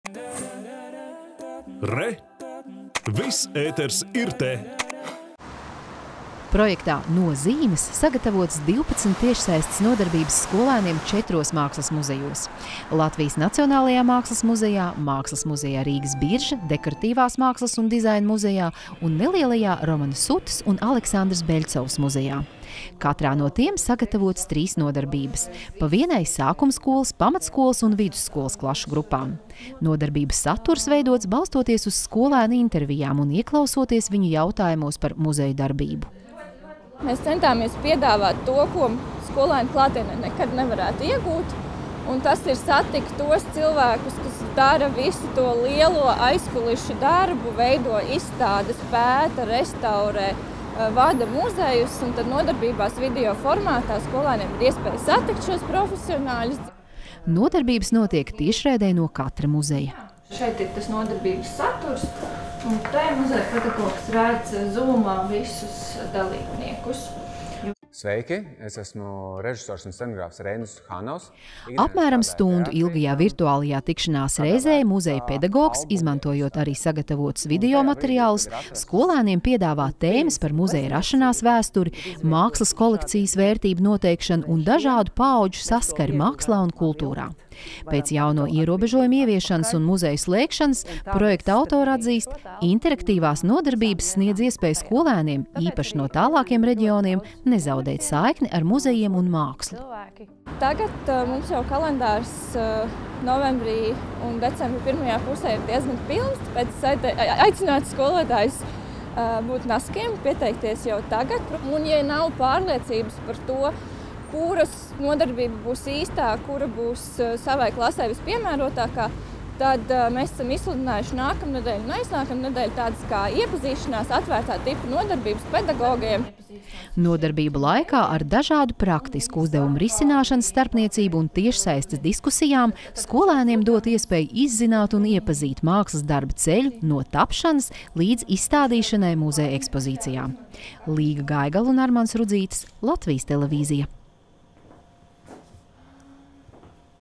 12 tiešsaistes nodarbības četros mākslas muzejos
Laikā, kad muzeji ir slēgti, īpaši aktuāls ir kļuvis Latvijas Nacionālā mākslas muzeja attālinātās izglītības projekts “No-zīmes”. Tas paredzēts skolēniem no pirmās līdz divpadsmitajai klasei, un tas ļauj ielūkoties četros Rīgas mākslas muzejos. Turklāt ielūkoties ne tikai izstāžu zālēs, bet arī telpās, kas apmeklētājiem nav pieejamas, piemēram, muzeju krātuvēs un restauratoru darbnīcās. Plašāk Līgas Gaigalas sižetā.